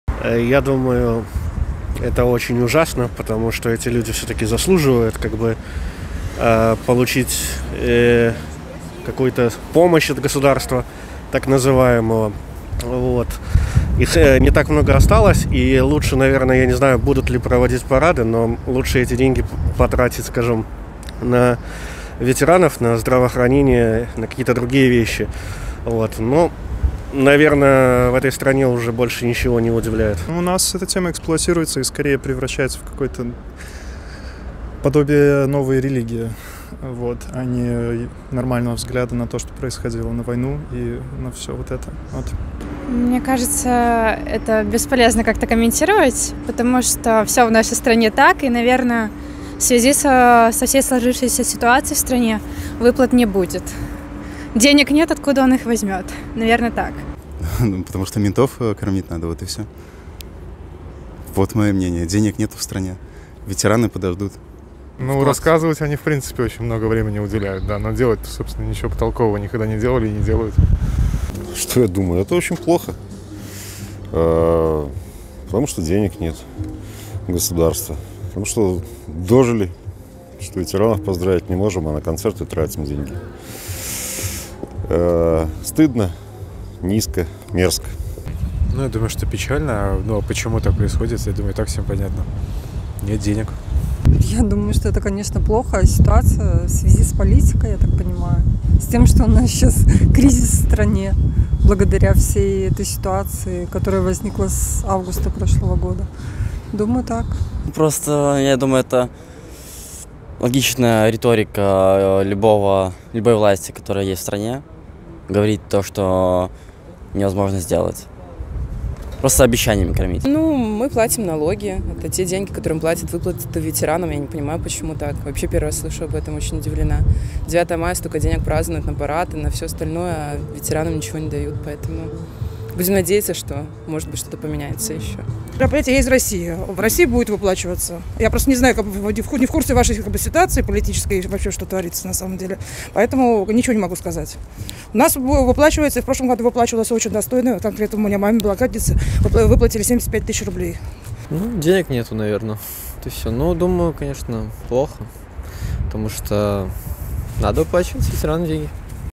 Почему ветеранов лишили выплаты к 9 Мая? / Опрос
Ветераны в Беларуси не получат ничего к 9 мая, это уже объявили официально. Как вы относитесь к этому?